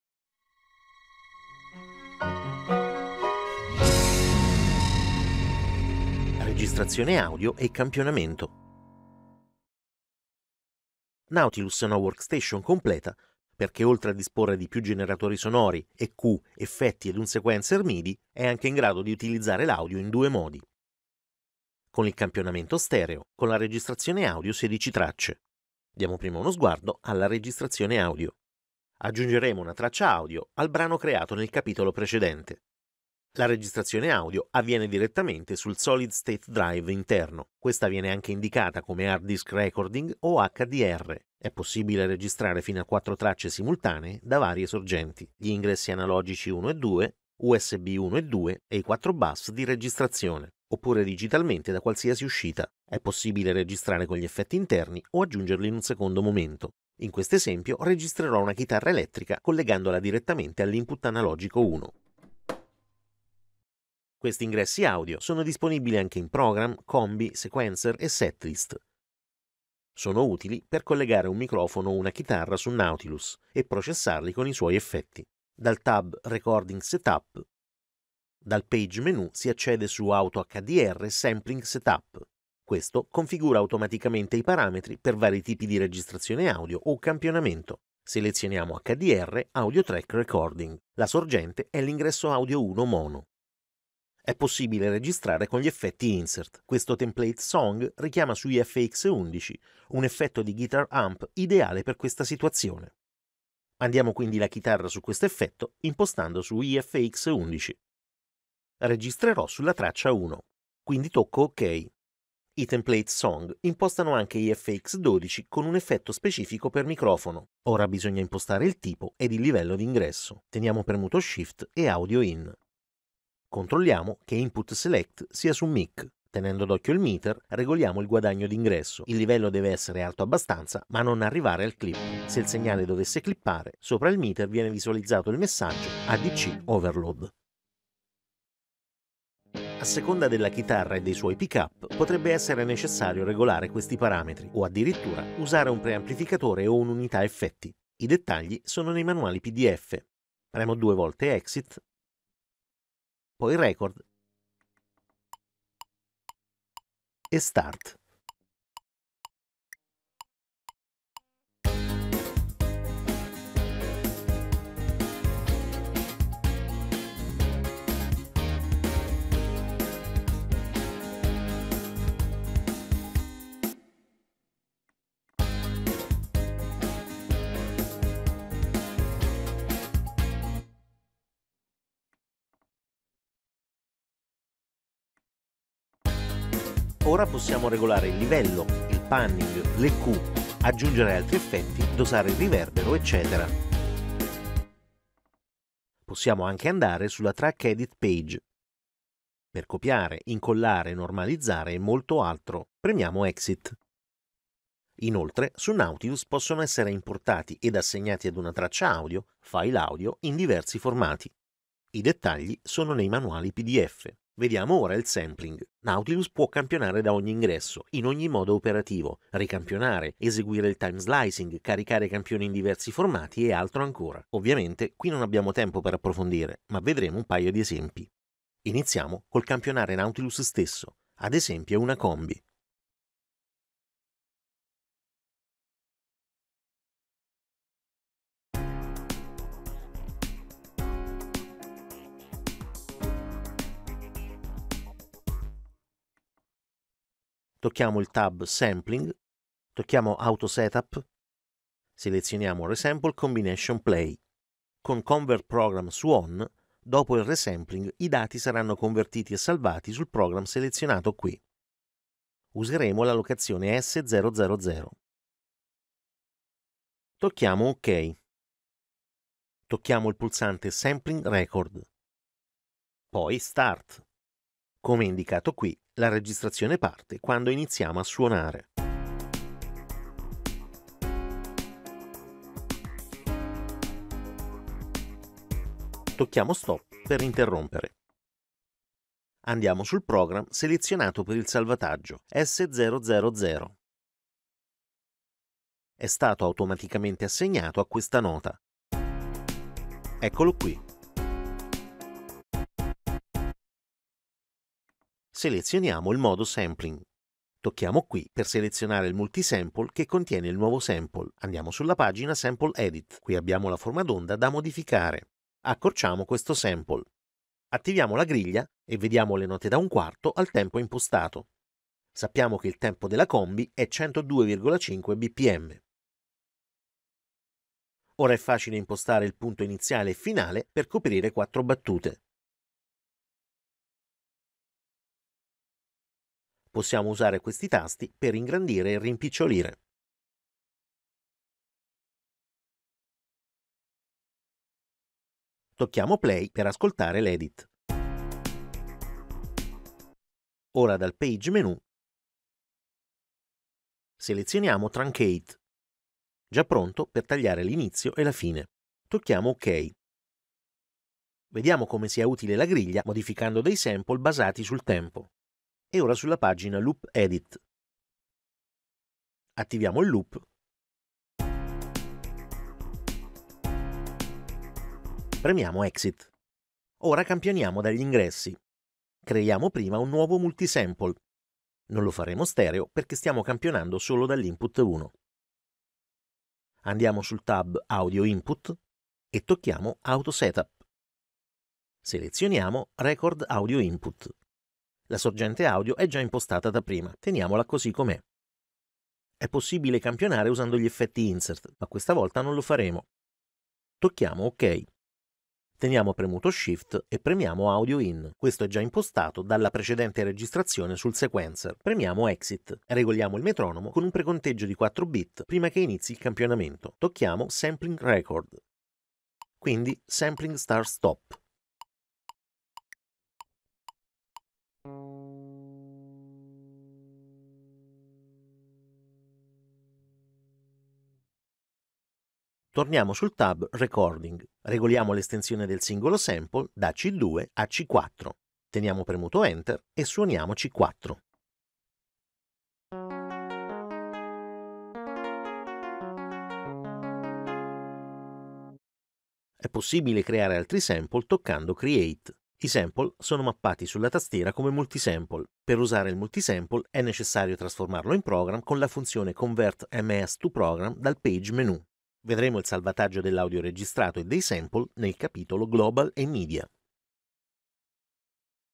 KORG NAUTILUS Video Manuale In Italiano Parte Sesta: Audio Recording & Sampling
Dettagliata spiegazione in Italiano delle principali funzioni ed operatività della Music Workstation KORG NAUTILUS.
In questo capitolo: Audio Recording & Sampling
Uso della Workstation come registratore audio multitraccia e Campionatore
Seguici anche su: